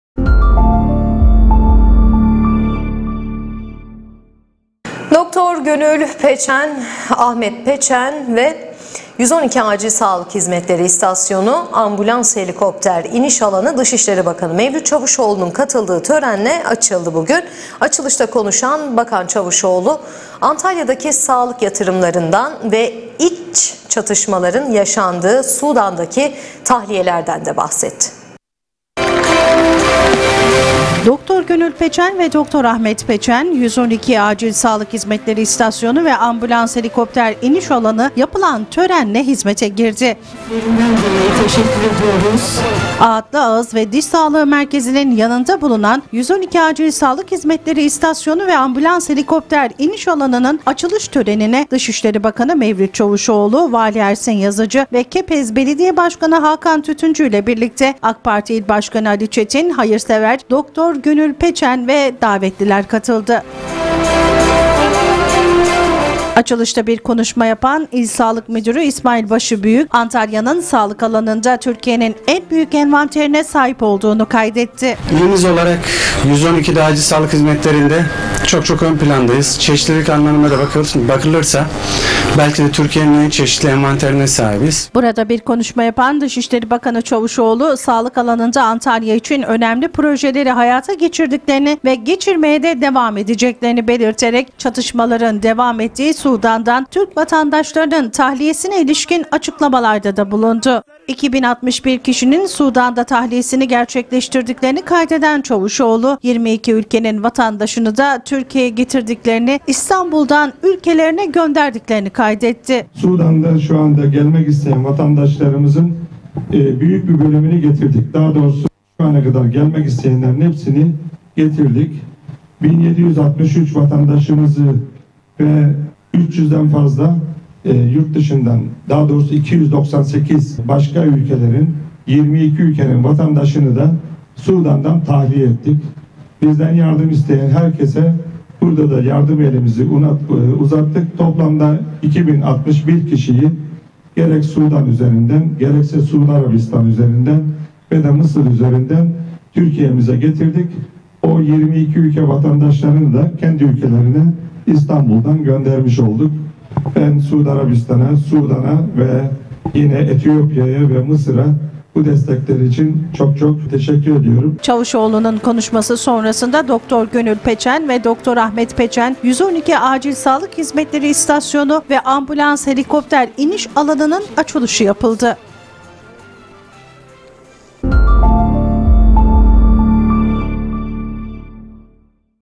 SUDAN'DAN 2 BİN 61 KİŞİ TAHLİYE EDİLDİ
DR. GÖNÜL PEÇEN-DR. AHMET PEÇEN 112 ACİL SAĞLIK HİZMETLERİ İSTASYONU VE AMBULANS HELİKOPTER İNİŞ ALANI, DIŞİŞLERİ BAKANI MEVLÜT ÇAVUŞOĞLU'NUN KATILDIĞI TÖREN İLE AÇILDI. ANTAYA'DA SAĞLIK HİZMETLERİNDE ÖNEMLİ BİR YERE SAHİP OLACAK İSTASYONUN AÇILIŞINDA KONUŞAN BAKAN ÇAVUŞOĞLU, ANTALYA'DAKİ SAĞLIK YATIRIMLARINDAN BAHSETTİ. ÇAVUŞOĞLU İÇ ÇATIŞMALARIN YAŞANDIĞI SUDAN'DAKİ TAHLİYELERİN DE TAMAMLANDIĞINI AÇIKLADI.